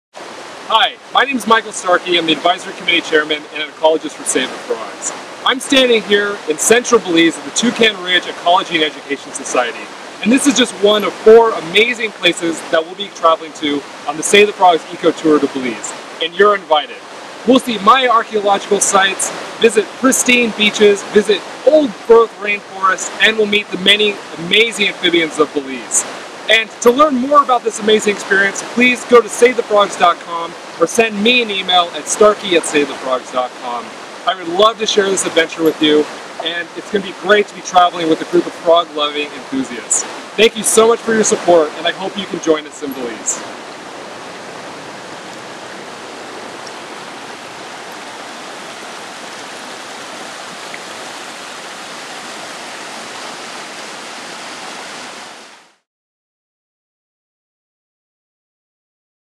Join Save The Frogs in Belize in July 2015
Please join us in Belize July 9th-18th, 2015!
Belize is a wonderful little country located east of Guatemala and nestled beneath the Yucatán Peninsula of Mexico. Like many countries in Central America, Belize is a hotspot of biodiversity. Belize is home to at least 37 amphibian species, as well as an array of bats, birds, lizards, corals and other amazing wildlife. The trip will be led by SAVE THE FROGS! Advisory Committee Chairman Michael Starkey, who has participated in, coordinated and led Belize eco-tours for 5 years! We will also be accompanied by SAVE THE FROGS! Founder Dr. Kerry Kriger, SAVE THE FROGS! Ecologist Kathlyn Franco, and Mayan naturalists and field guides. Group size is between 10 and 19 participants. Our SAVE THE FROGS! 2013 Ecotour & 2014 Ecotour were huge successes and we fully expect 2015 to be just as amazing. Eco-Tour and have the time of your life!

"I did this trip in 2014...absolutely FABULOUS!"
-- Rebecca Buell

Please watch our newest video:
"LA Times Interviews SAVE THE FROGS! Founder Dr. Kerry Kriger"